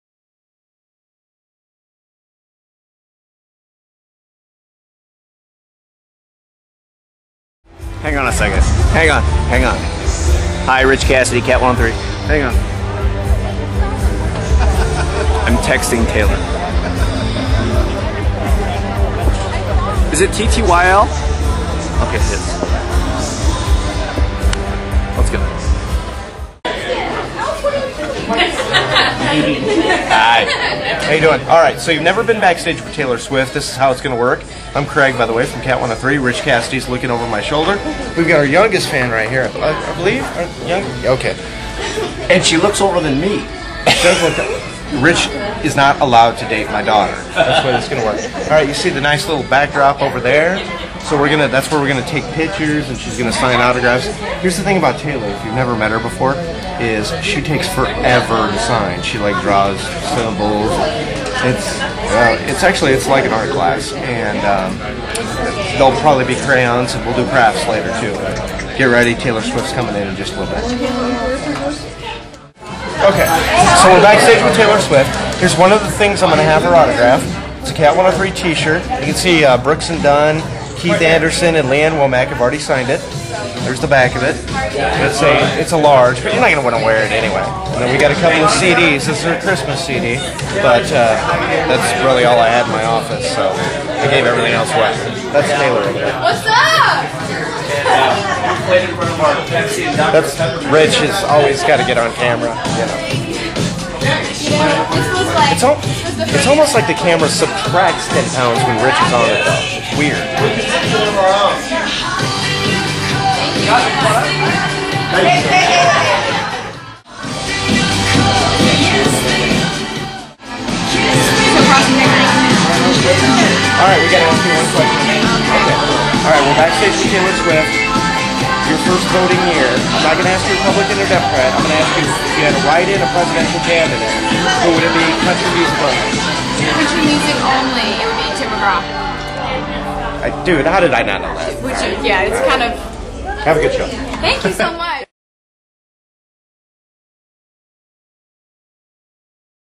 Backstage with Taylor Swift
Join Craig Allen and Ritch Cassidy as we head back to see Taylor Swift!